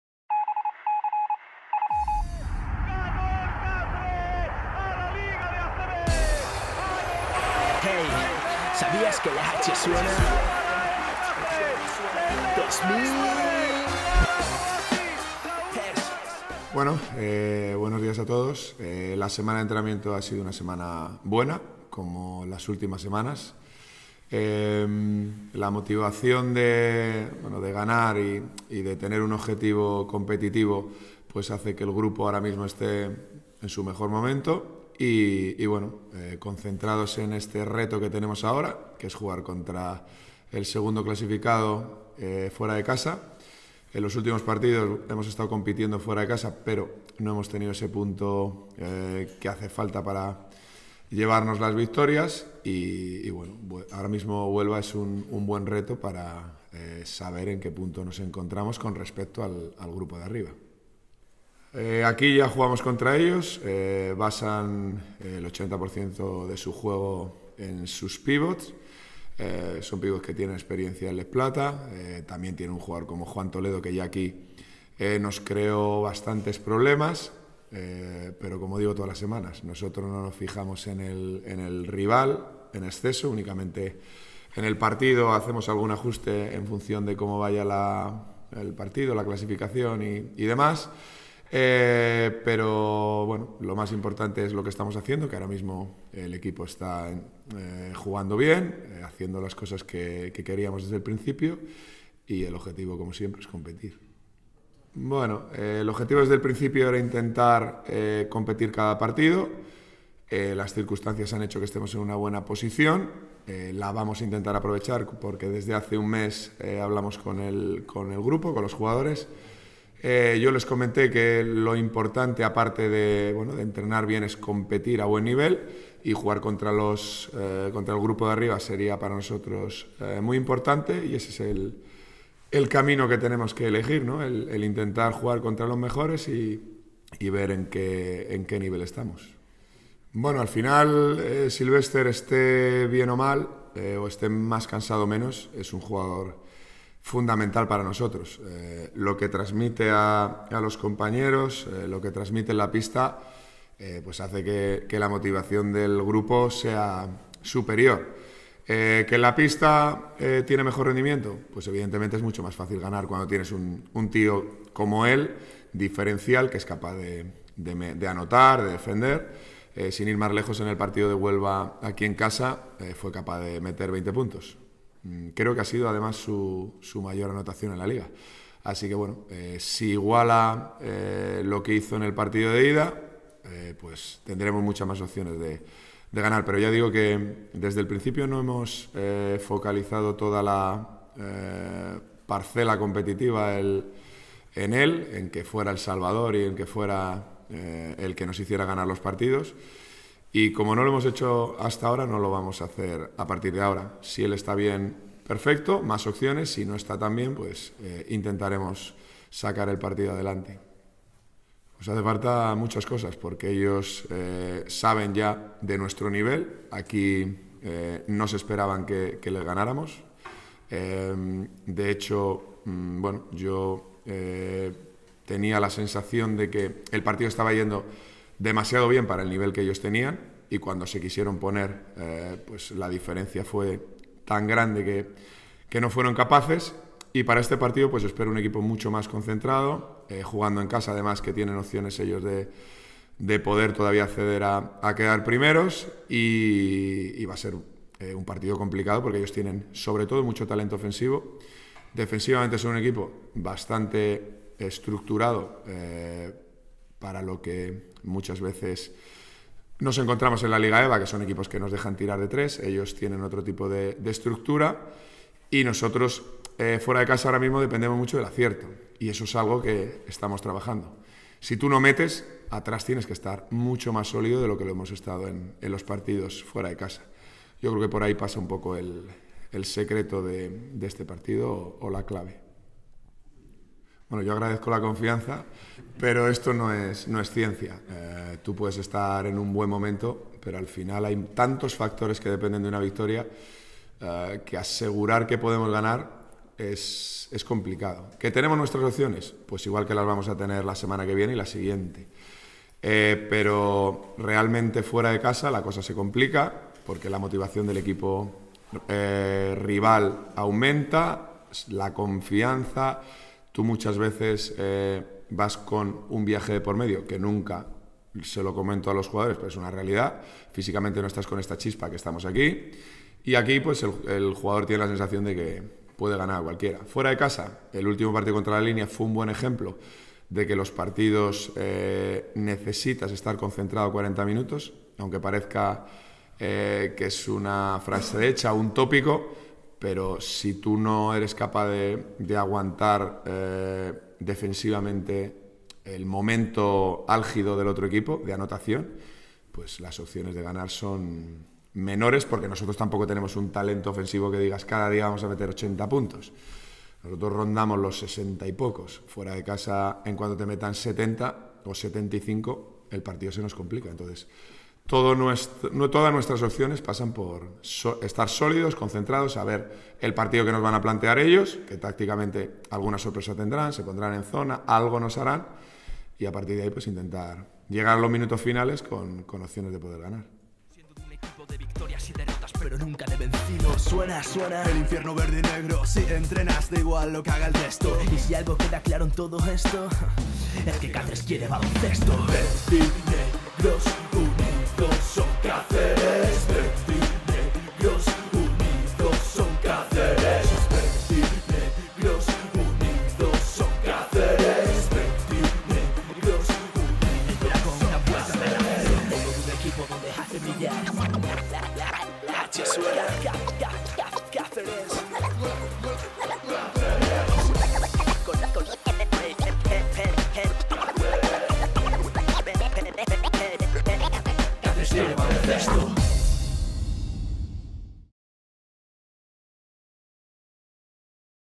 t1920pj16 EBA armando gomez huelva torta
Caceresbasket 2019/20 El entrenador del Torta del Casar Extremadura, Armando Gómez, comparece ante los medios de comunicación, en la previa de la jornada 16 de la Liga EBA, que enfrentará a su equipo este domingo a las 12:00h, con el Huelva en el Polideportivo Andrés Estrada.